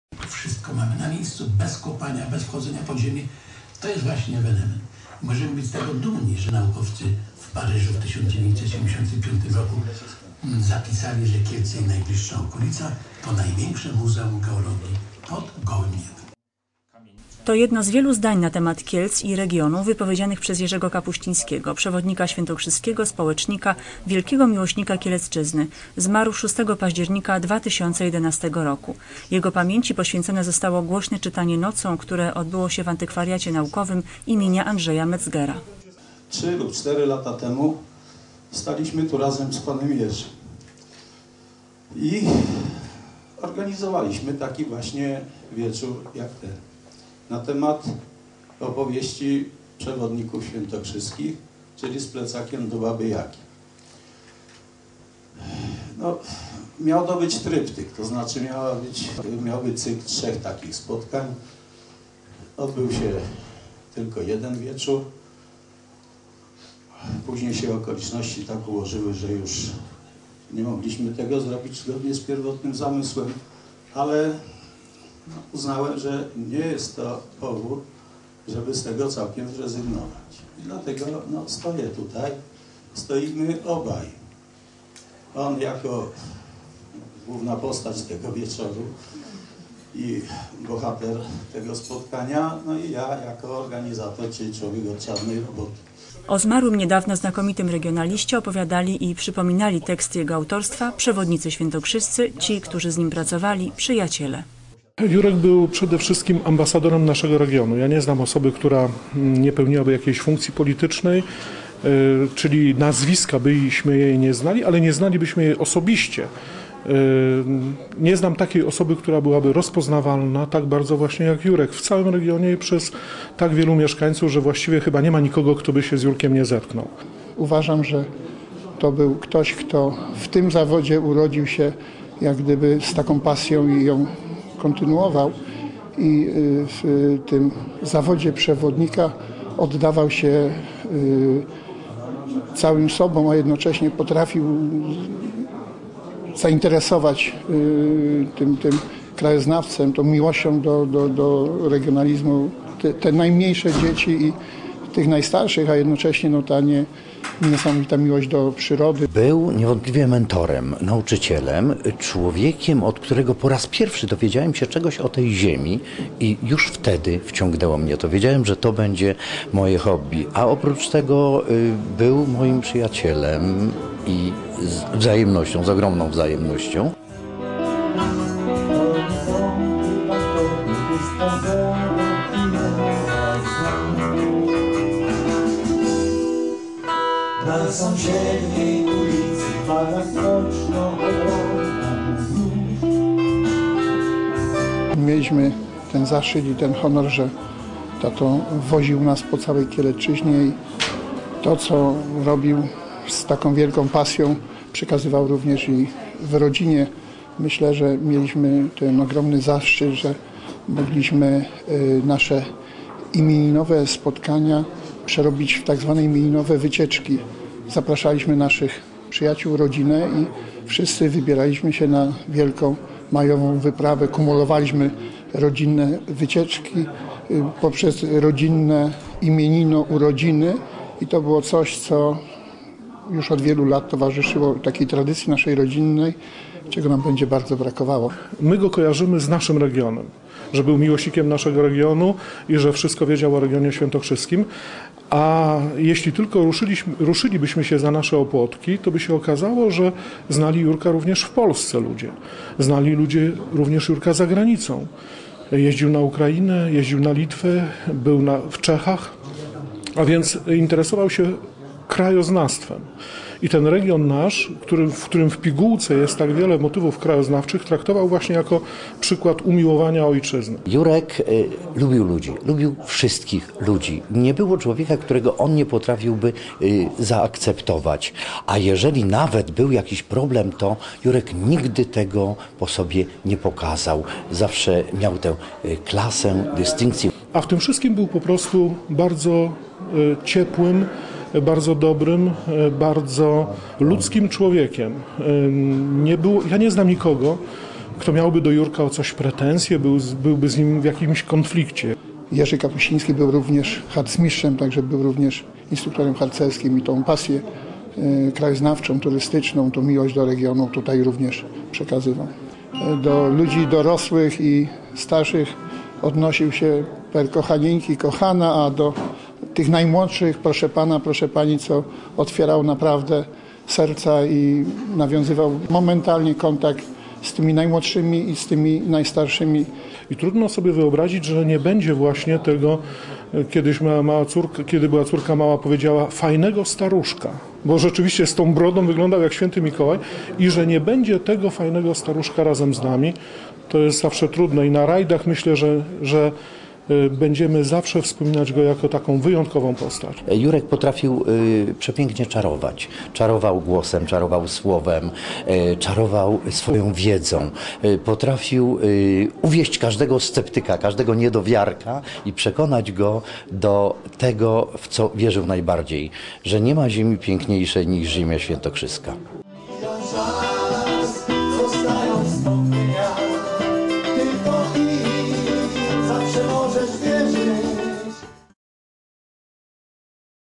Wspomnienie Jerzego Kapuścińskiego
Był przewodnikiem i piewcą Ziemi Świętokrzyskiej, jej wielkim ambasadorem, honorowym członkiem PTTK, przyjacielem dzieci i młodzieży. Jako jeden znielicznych potrafił zaszczepić wśród swoich wychowanków potrzebę miłości do „Małej Ojczyzny", zachęcając jednocześnie do interesowania się innymi regionami Europy. Kielce uhonorowały jego działalność Nagrodą Miasta za 1998 r. Jerzy Kapuściński - zmarł 6 października 2011 r.
Jego pamięci poświęcone zostało "Głośne Czytanie Nocą", które odbyło się w Antykwariacie Naukowym im. Andrzeja Metzgera.